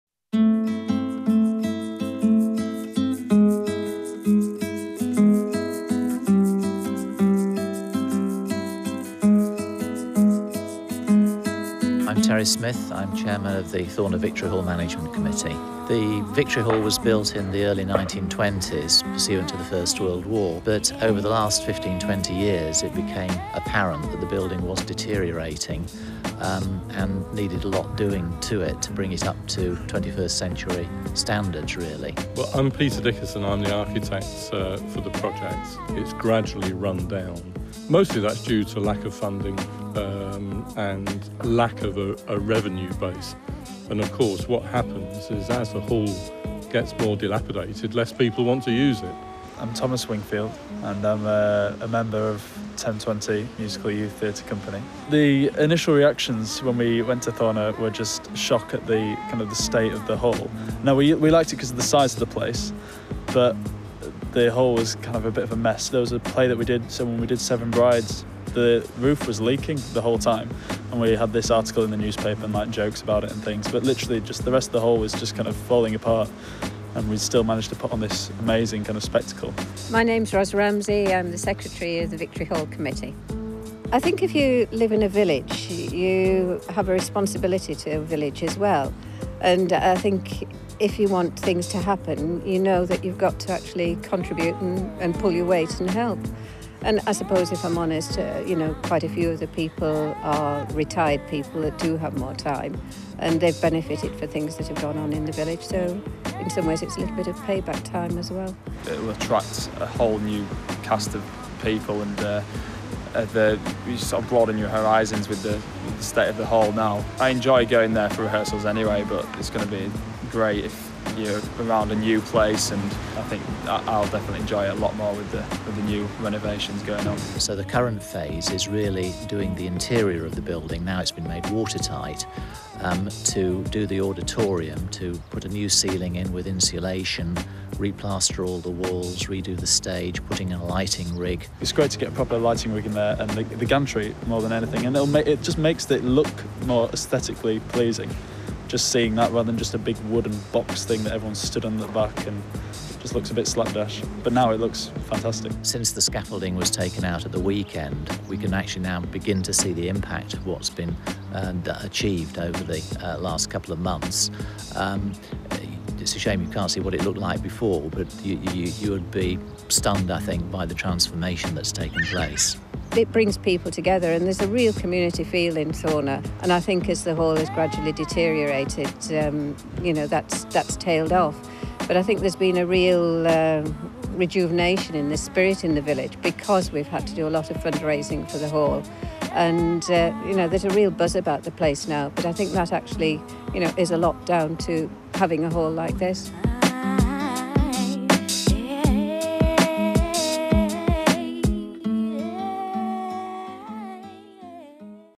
Extraordinary Story 1 - Thorner Victory Hall
Community groups across Thorner village benefitted when Thorner Victory Hall received a head to toe makeover. Benefitting over 4,000 people, the refurbished hall has brought a buzz to the village. The achievements of the group were beyond expectations and will now make a real difference for generations to come.